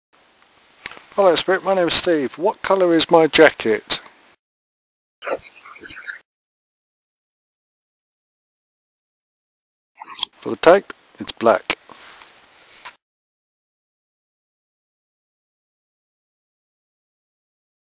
Bicknor Woods real ghost voice EVP - six
Bicknor Woods real ghost voice recorded during our ghost hunt at Bicknor Woods, Kent on 18th January 2013. You hear a person ask a question and then the response by the ghost/spirit